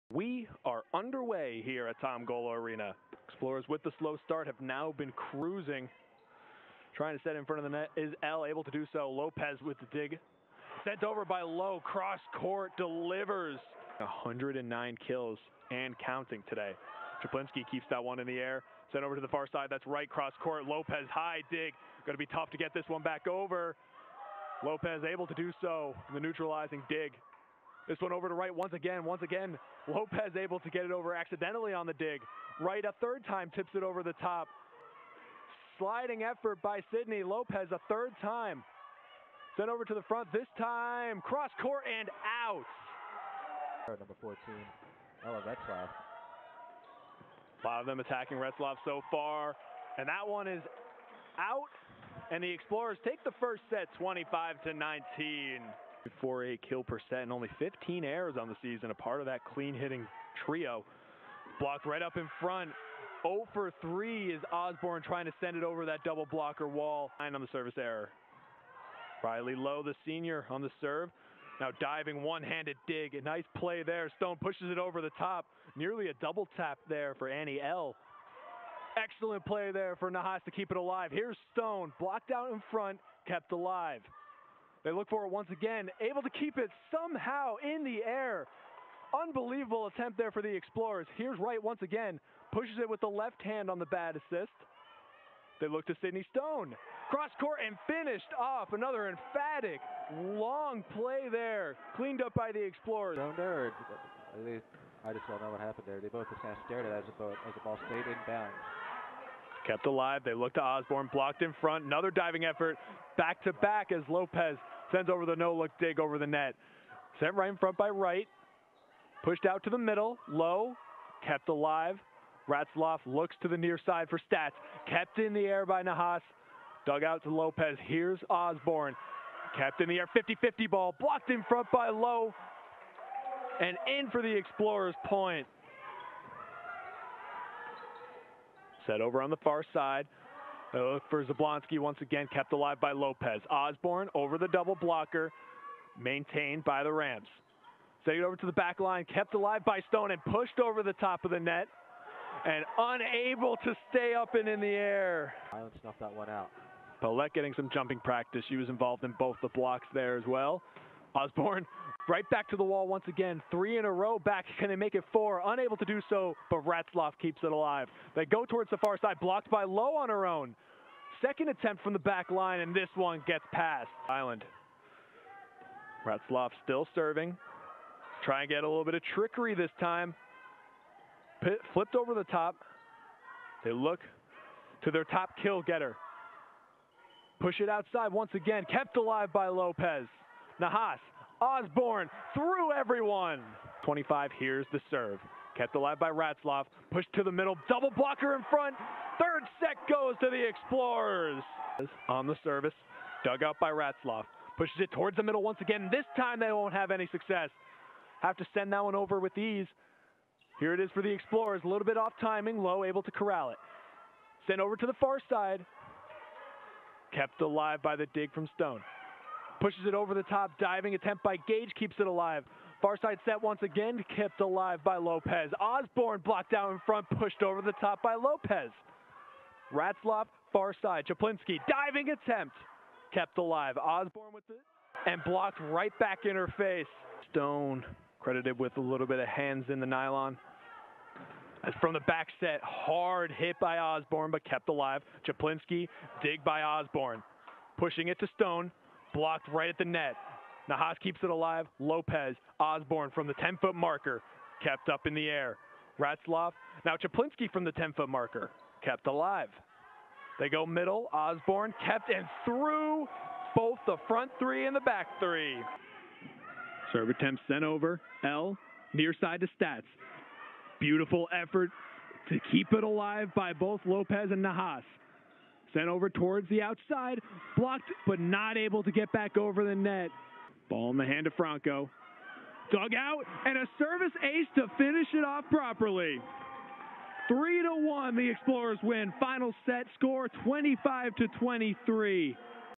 URI vs La Salle Highlights
The undefeated La Salle Explorers hosted their A10 foes, the URI Rams, for a must-watch event at Tom Gola Arena. Check out the extended highlights!

*All videos and images courtesy of ESPN+ and the NCAA